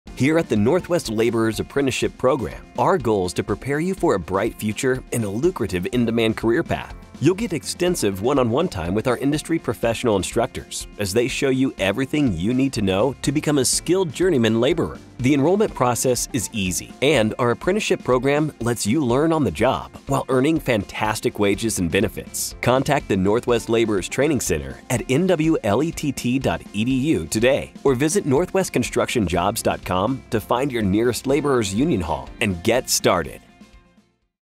NWLETT Earn While You Learn V3
Build your future as an apprentice with the Northwest Laborers! Earn while you learn - and build a career with great benefits while doing so.